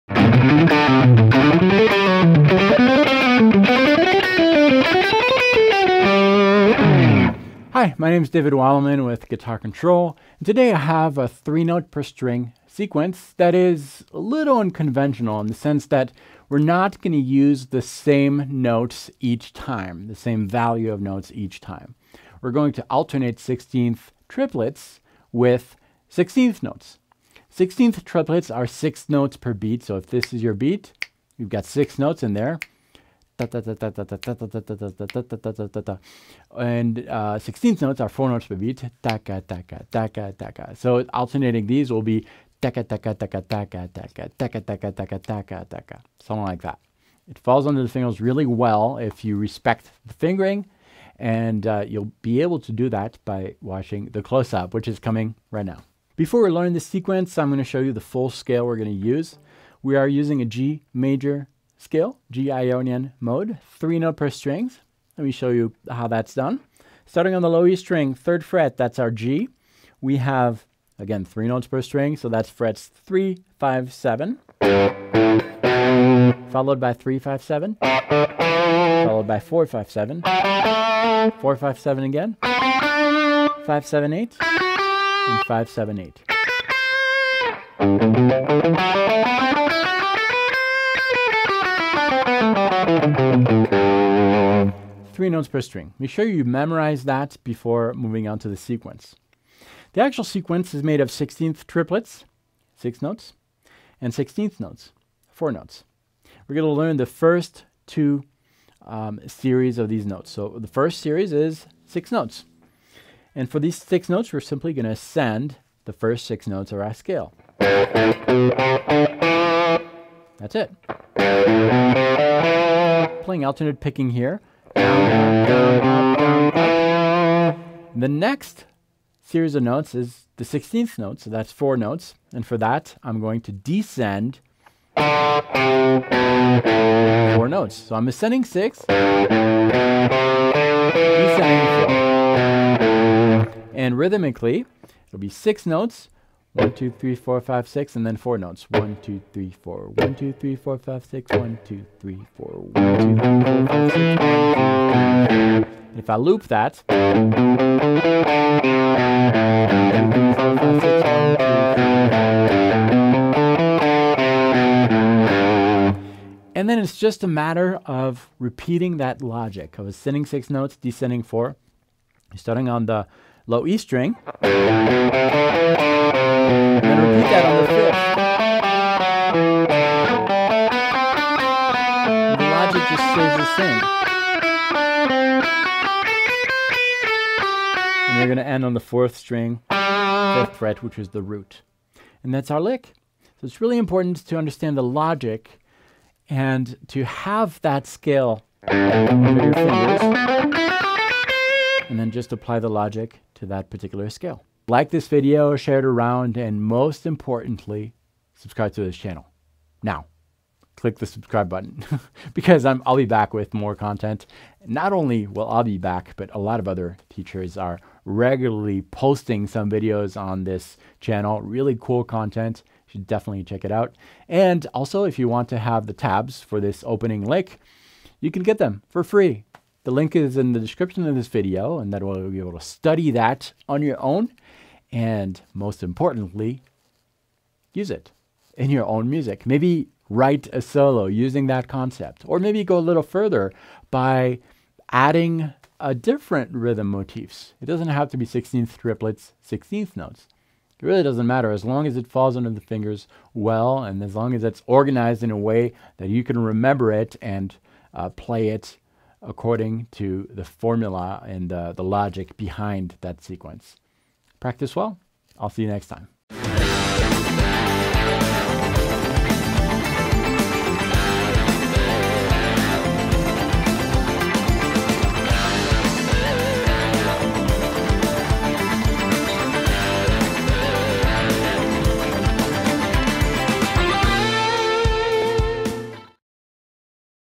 Easy Lead Guitar Lesson on Scales Sequences with Rhythm Alterations
Click in the link below to GET the Tabs!

Rhythmic structure is an important element of any musical endeavor. The rhythmic variation you apply to any series of notes or chords greatly affects the outcome of the musical passage. The focus of this video is to demonstrate how to vary your rhythm by alternating rhythmic note patterns to add color to your playing. Using G major as an example, David demonstrates the scale played in an uninteresting pattern used frequently when practicing scales, then he introduces two sixteenth note figures that use different rhythmic structures. The pattern is based on a six note ascending and four note descending pattern that introduces triplets as a rhythmic variation. By paying these two patterns in alternating style, what  you get is an interesting lick that can be taken and applied in any other key and used in the same way. Ideally, this is a jumping off point for you, and once you get the feel of the alternating rhythms introduced here, you can create your own rhythms to experiment with. Practicing this exercise and creating your own can add interest to your scale practice sessions and will greatly expand the way you improvise. As David does frequently, he discusses the logic behind the licks he teaches and stresses the importance of pattern. This licks, and any you create, can be moved from key to key or played in different positions on the fretboard to expand your knowledge of your instrument and help you to achieve better results in playing or composing situations. As with any lesson, take your time and watch first, then sit back and apply what you’ve seen. Once you’ve mastered that much, try getting creative and writing your own rhythmic patterns using the rhythm alteration method. You’ll be surprised how quickly you’ll be putting this stuff into practice in real-world playing situations and how much it will improve you as a player.